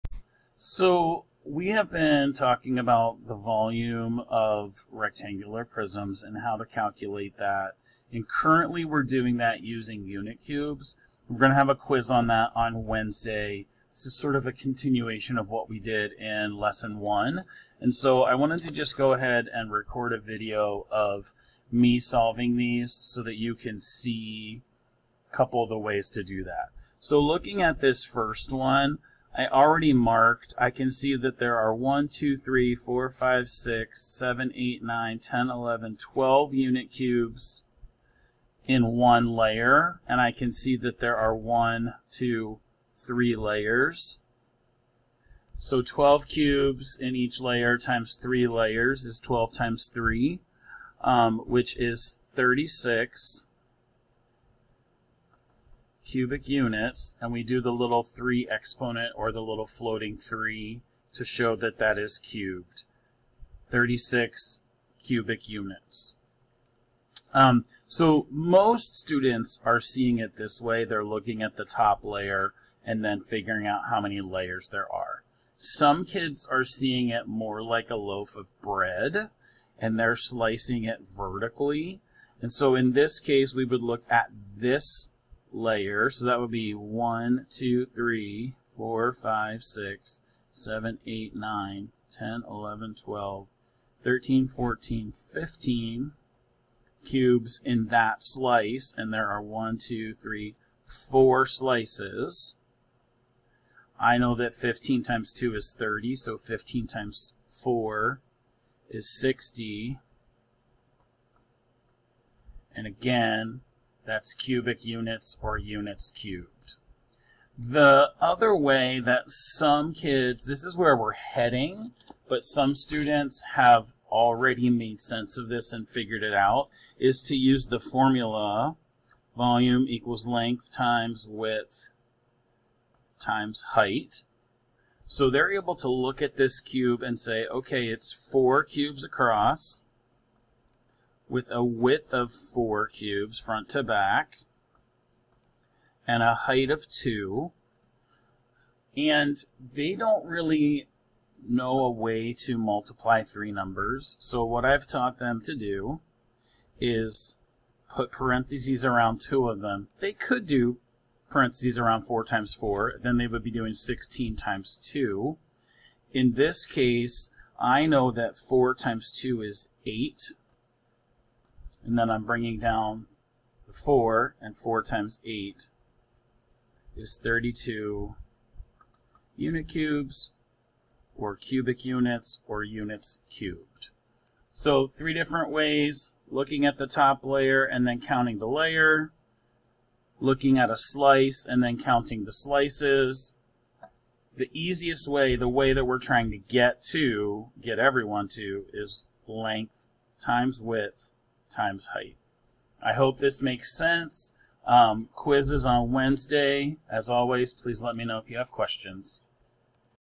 Volume of a Rectangular Prism (Unit Cubes)
Using unit cubes to find the volume of a rectangular prism